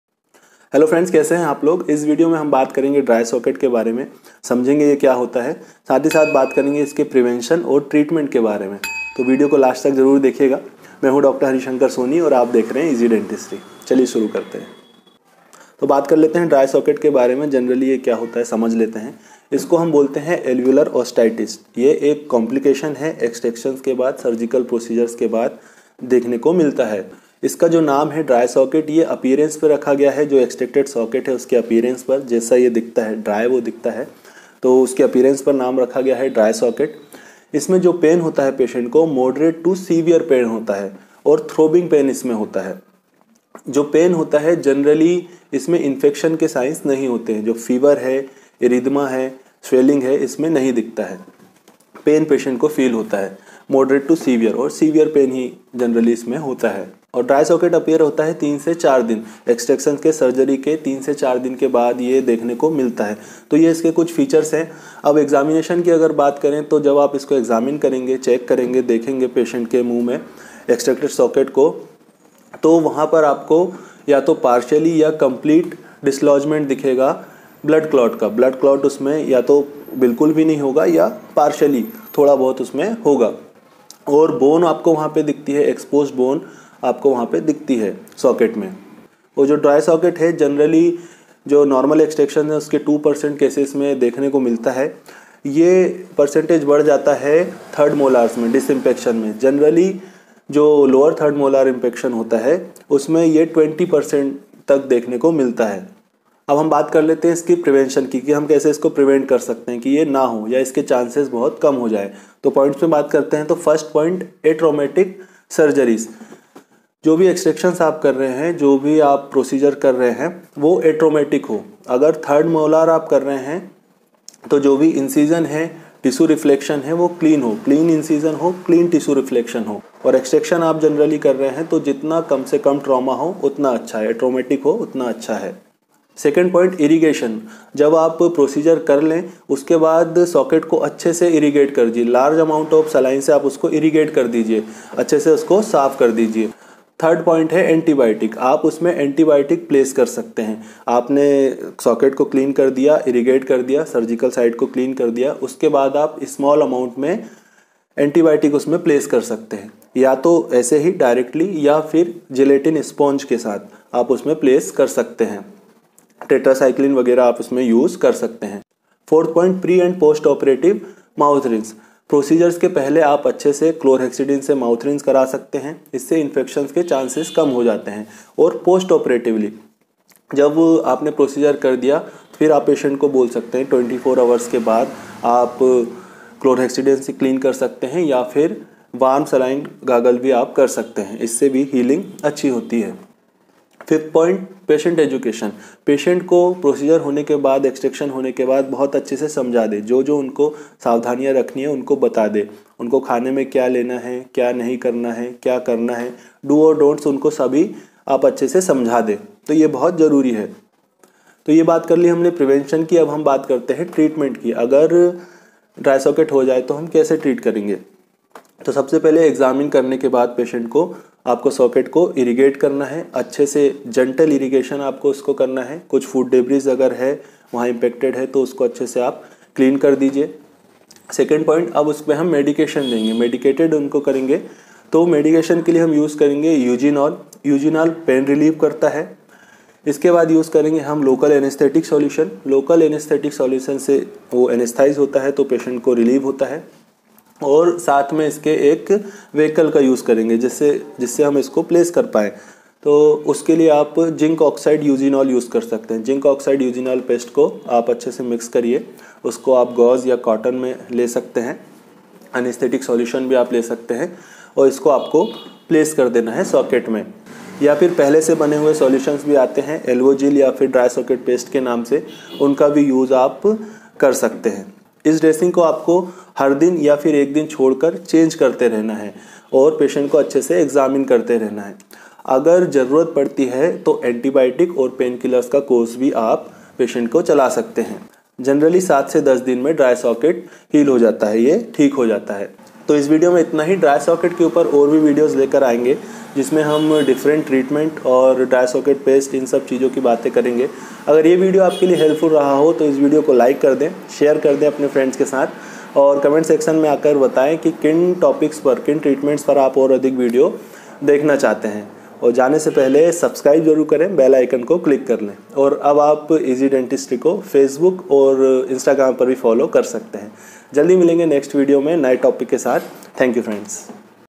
Dry socket management | Prevention & Treatment
Hello friends
This video is about dry socket
This a complications after extraction/surgical procedure.

Also known as Alveolar Osteitis..

Apeereance- extracted socket is dry , bone exposed and generally signs of infection is absent ( fever, infection , erythema)

Pain- moderate to severe , throbbing pain